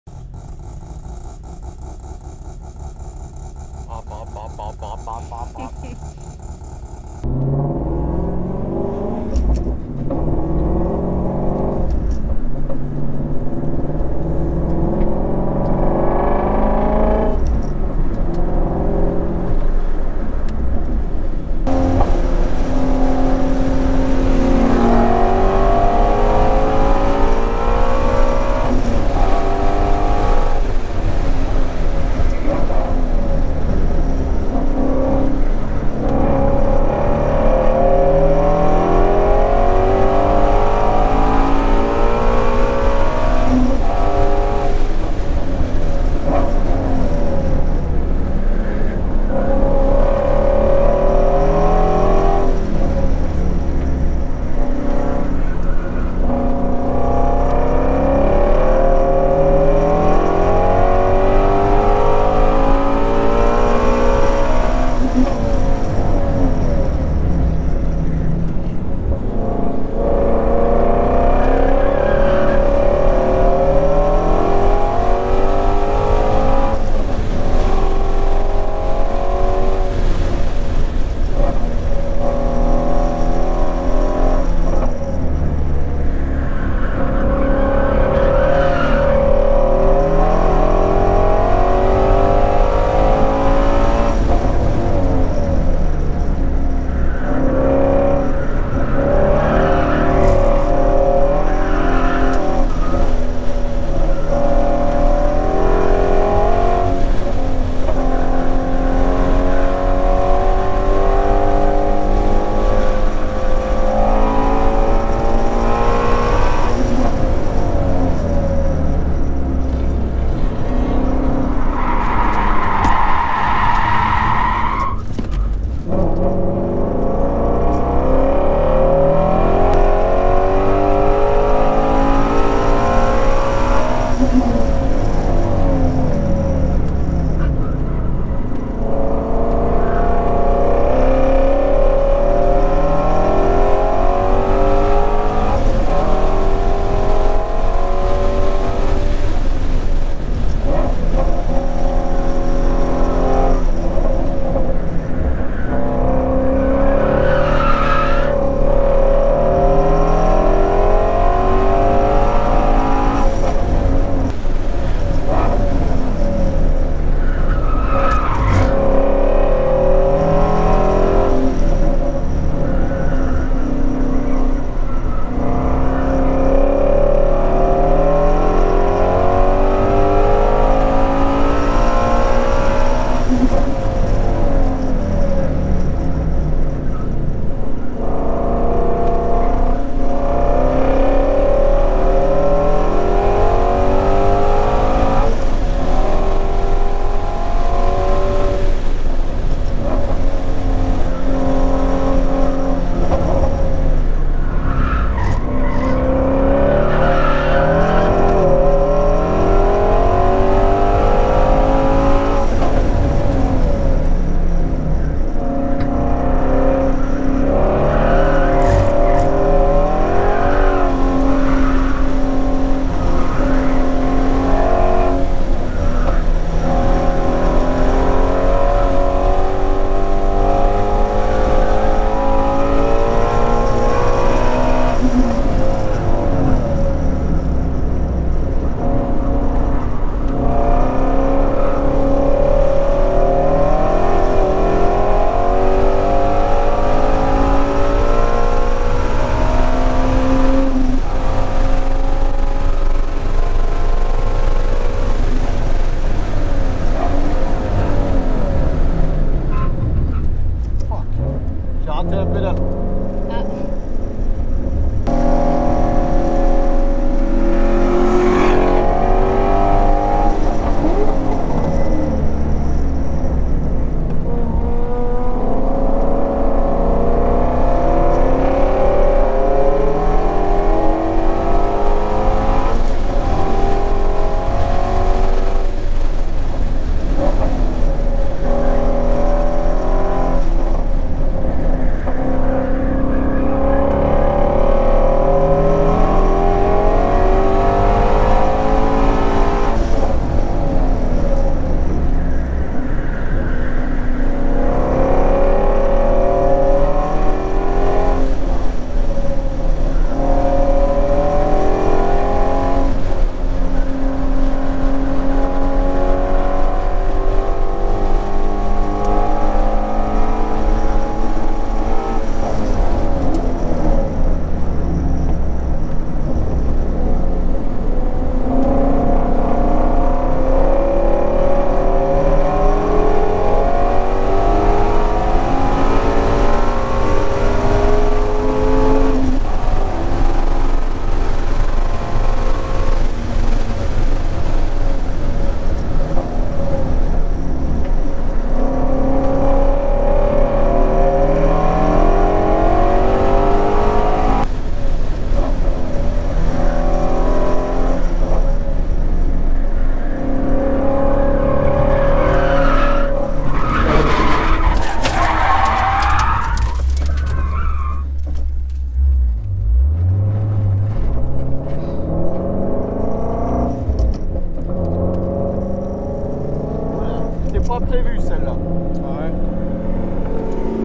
2017 Shelby GT350 at Gingerman Raceway - July 2019
It's been a while since I've been on track and several things changed on the car :
- Corbeau carbon bucket seats
- Schroth 6 point harnesses
- rear seat delete
- Ford Performance lowering springs
- Optimum Performance 25mm spacers in the front
- Full Tilt Boogie Racing adjustable toe links and camber rear arms
- BMW cradle & diff bushing inserts

The tires are still the original that came with the car in March 2017, so it has 295/35/19 Michelin PSS fronts and 305/35/19 Michelin PSS rears. Honestly, they were almost corded, had barely any tread left, and were heat cycled too many times, which combined with the more agressive and reactive chassis setup, made for a very slippery car to drive.

We had good fun though, weren't the fastest, weren't trying to be anyway, went sideways a few times and finished the day without any major incident.